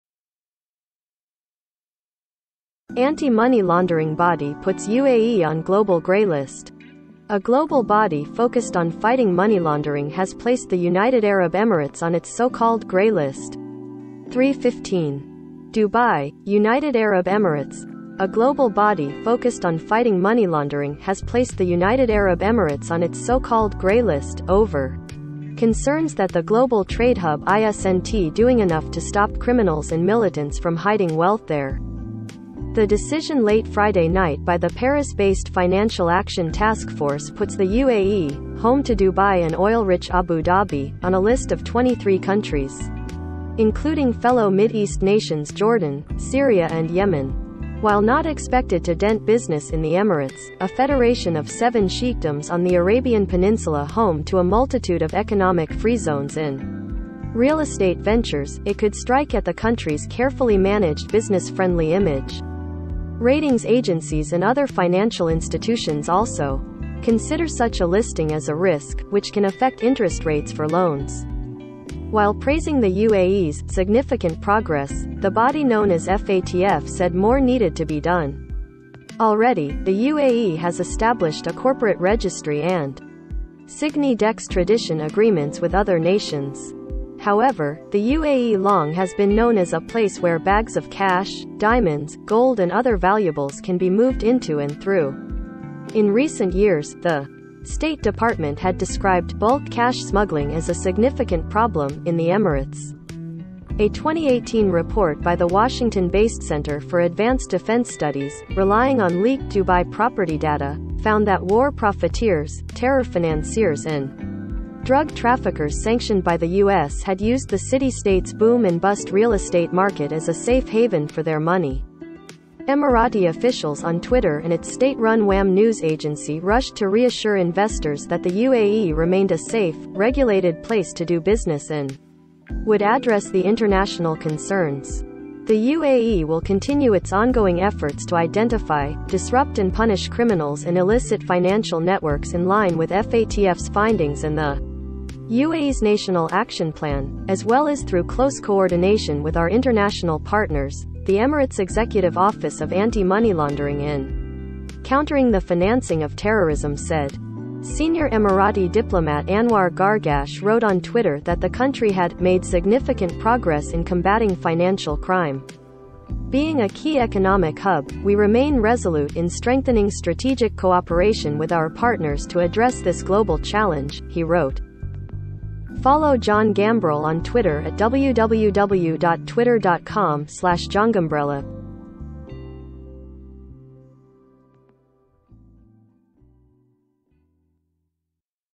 Anti-money-laundering body puts UAE on global 'gray' list - ABC News
Anti-money-laundering body puts UAE on global 'gray' listA global body focused on fighting money laundering has placed the United Arab Emirates on its so-called “gray list.”3:15DUBAI, United Arab Emirates -- A global body focused on fighting money laundering has placed the United Arab Emirates on it credit